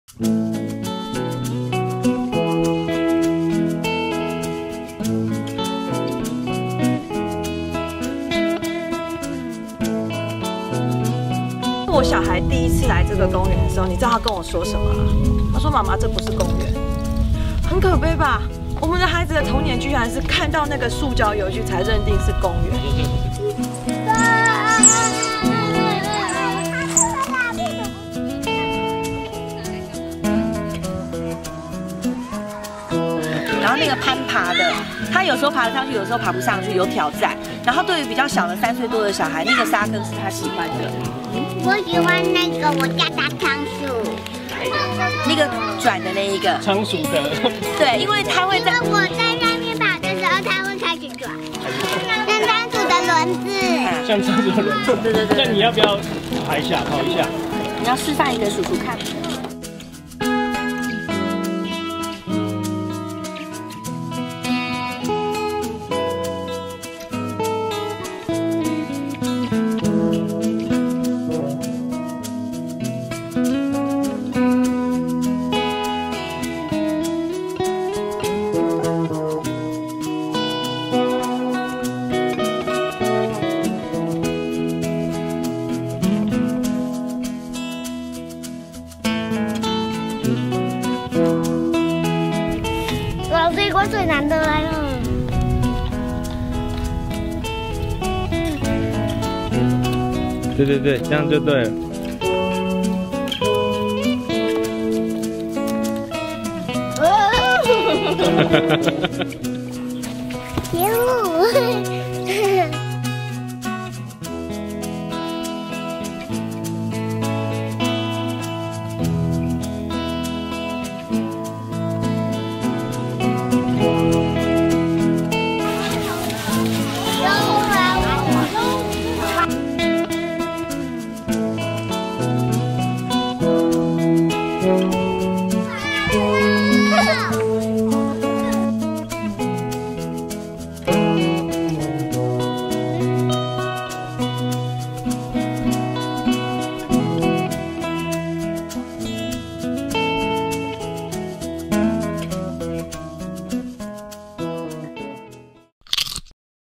全台最強公園 - 鰲峰山
蘋果調查全台罐頭公園亂象，也記錄下特色公園，發現台中鰲峰山公園設備完善，並擁有多樣性的遊具，結合自然風景等特色，堪稱全台最強公園，放假，就帶著孩子去玩耍吧！